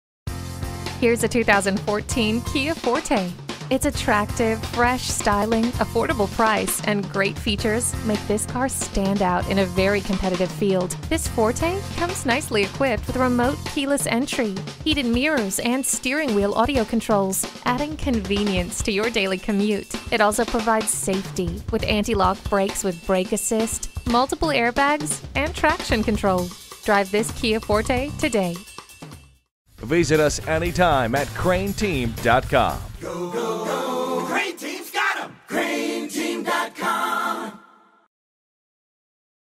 2014 Kia Forte Sherwood AR Little Rock, AR 4KC7731 - SOLD
Year: 2014
Make: Kia
Model: Forte
Trim: SED LX AT
Engine: 1.8L I4 DOHC D-CVVT Engine
Transmission: Automatic
Color: Abyss Blue
Interior: Gray
Stock #: 4KC7731

Address:
5830 Warden Rd